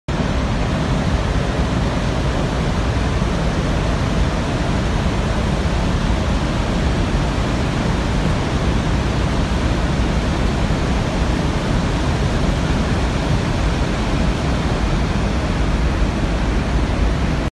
SARDAR SAROVAR NAMRADA DAM-17/08/2022(1)
mp mathi full pani avi rahyu 6